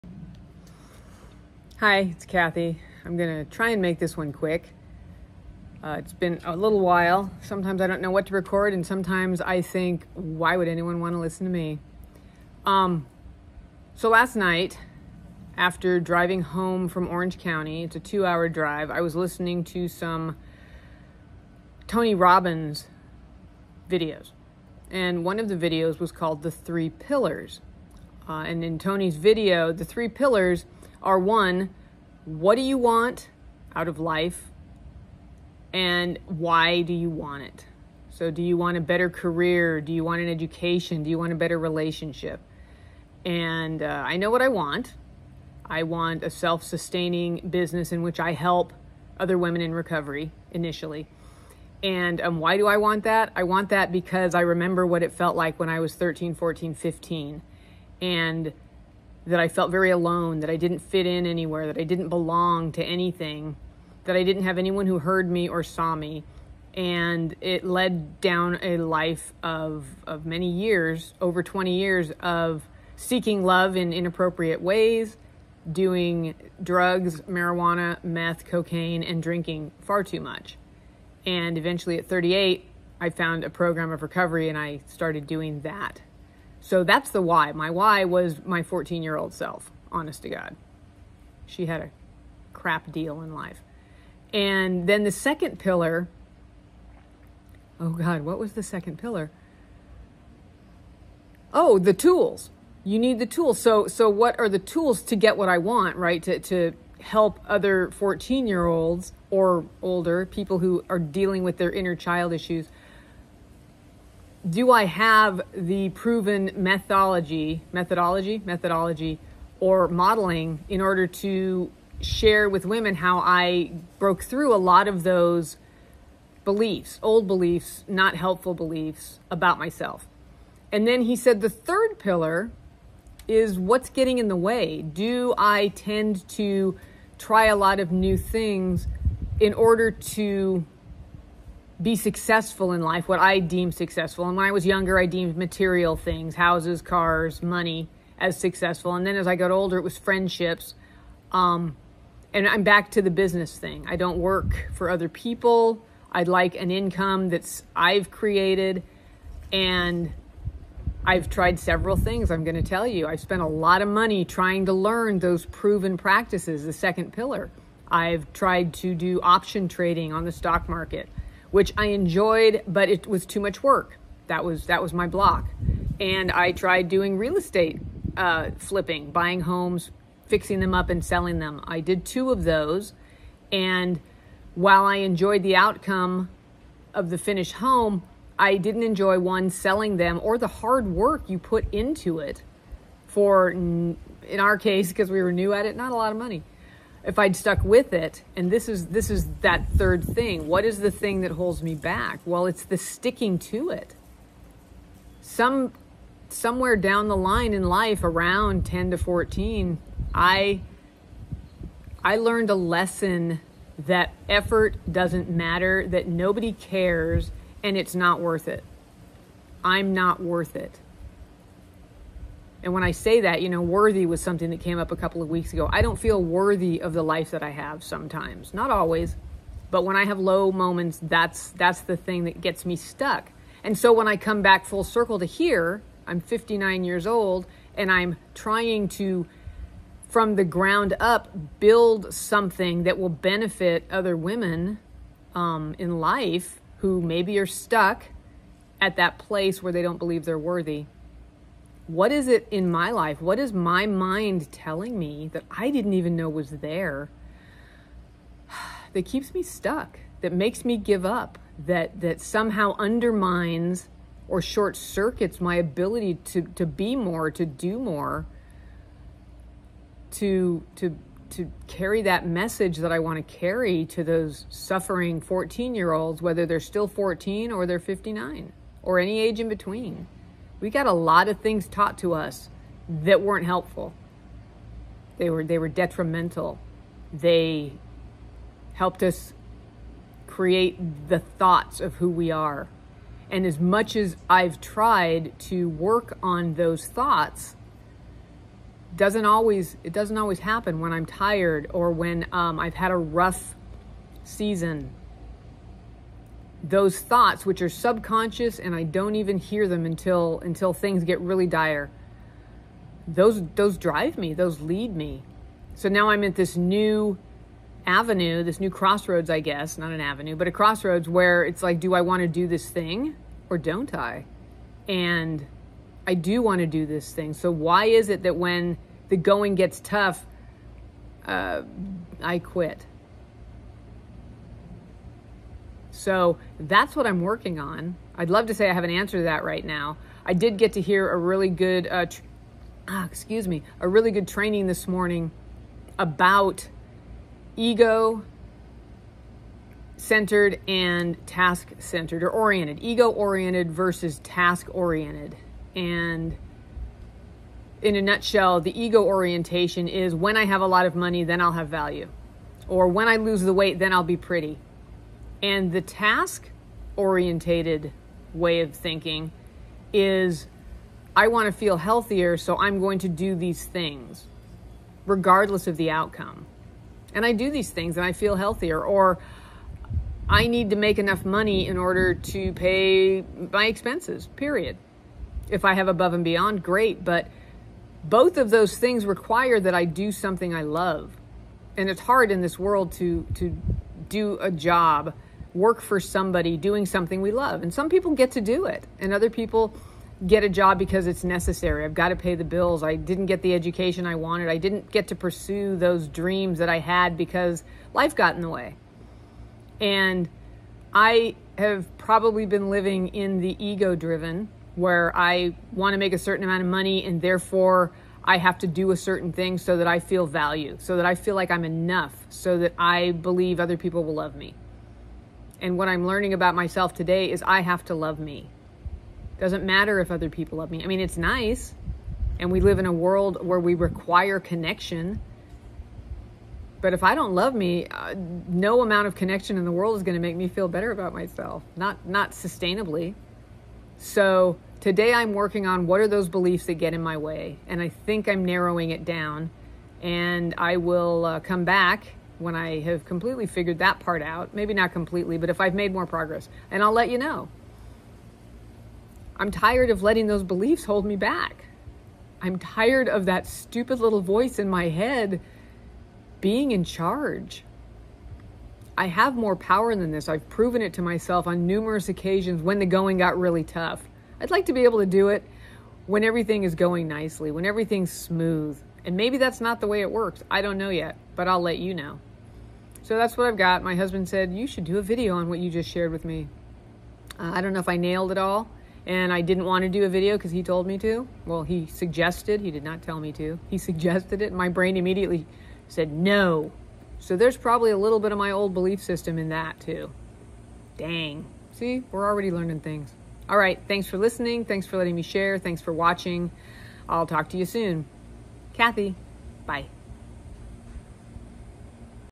Hidden Beliefs, Visible Impact
What holds us back? In life, in relationships. Have you ever signed up for something - a course, or a workshop, or an evening out - only to bail out at the last minute or halfway through? Me too. Today I talk about the third of Tony Robbins Three Pillars and how I’m working towards uncovering those old, hidden UNTRUE beliefs I hold as true that keep me from making next level changes in my life - particularly in the area of income.